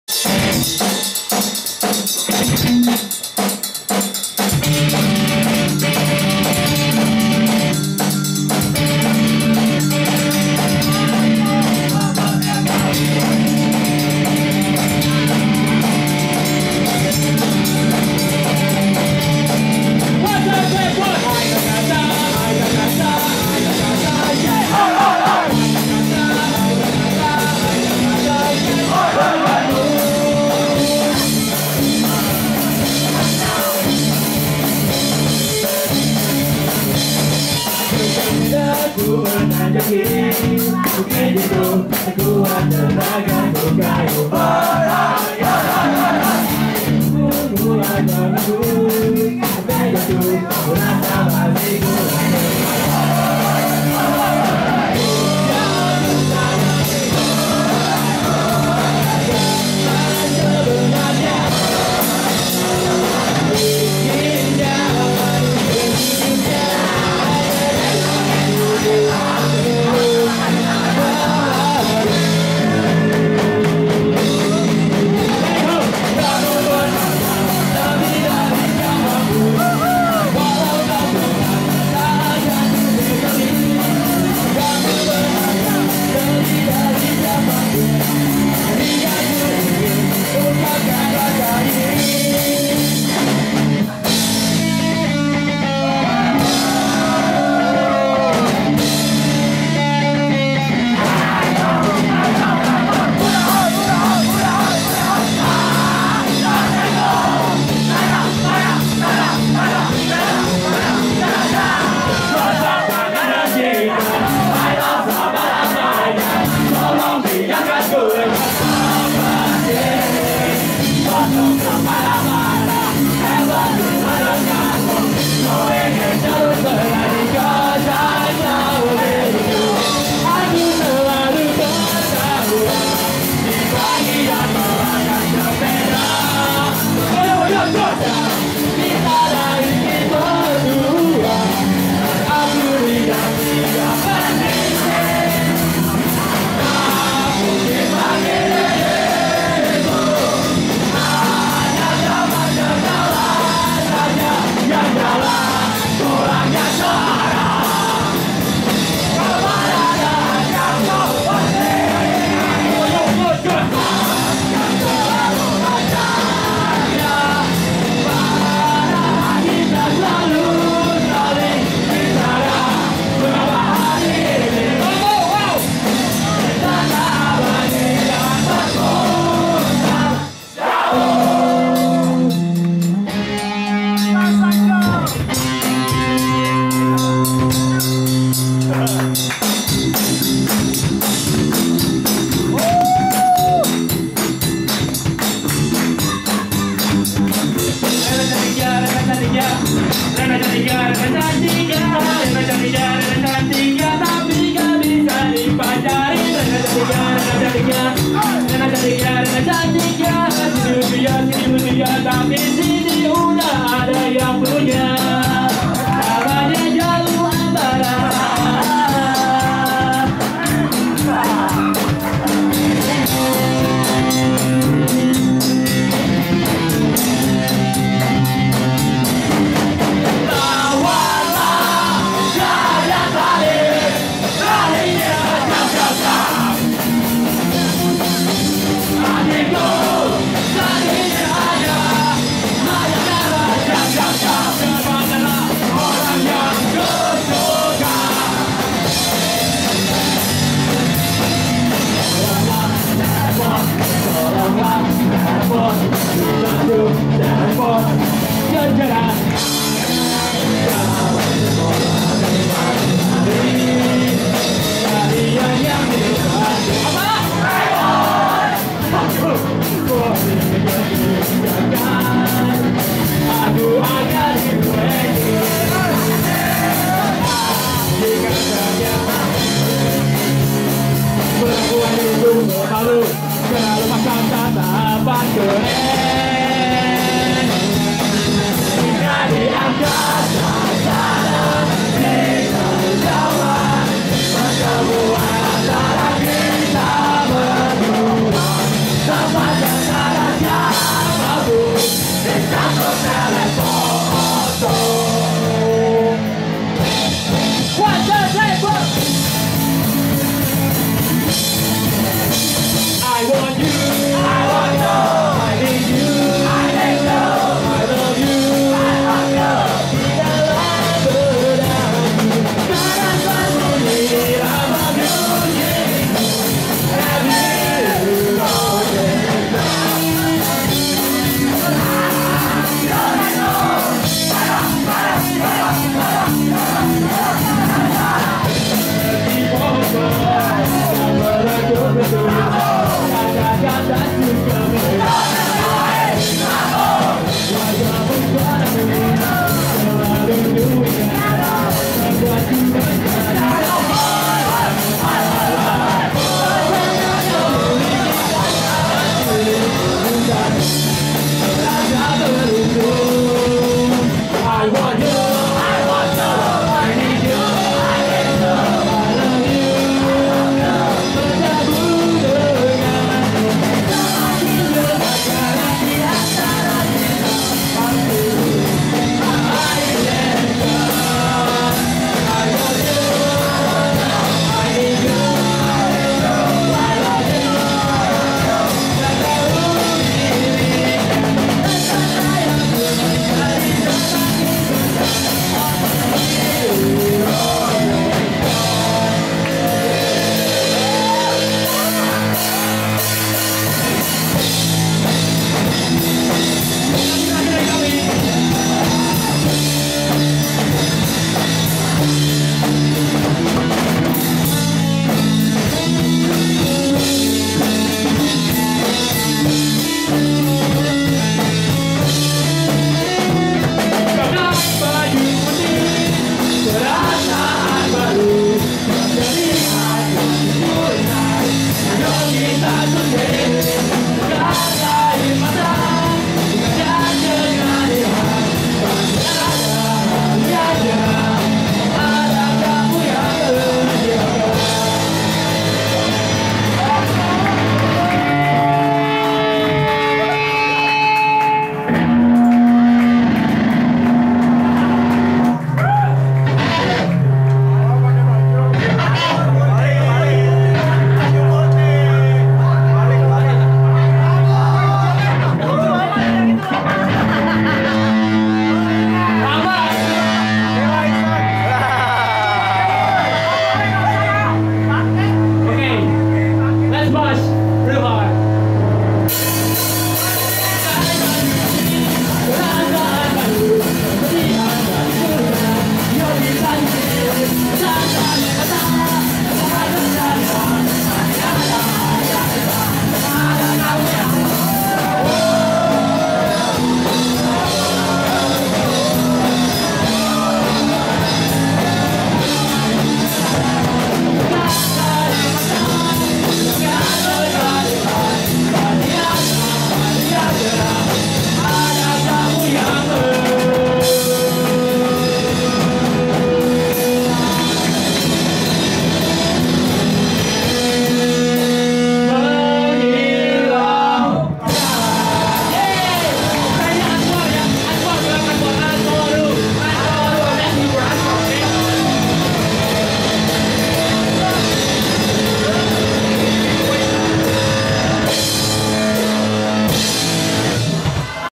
Bingo0202 : Saturday Night Karaoke (Part 8)
Saturday Night Karaoke at Bingo0202 event, in Demajors Gandaria. 2 February 2013.